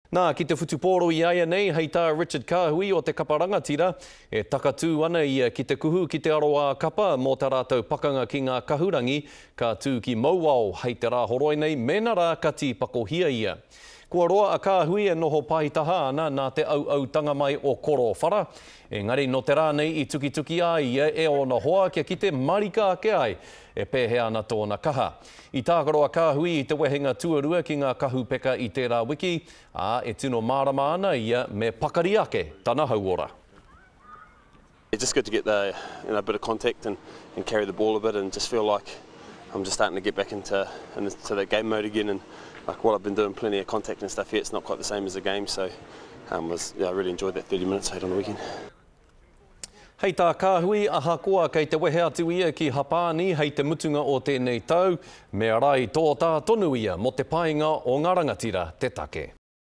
Kahui primed for return to Chiefs
The Chiefs' centre Richard Kahui looks ready to start for his side's next match against the Blues in Mt Maunganui on Saturday if he's selected. After a long spell away due to injuries, his team mates have welcomed him back into the fold with some hard contact training today, helping him build his confidence.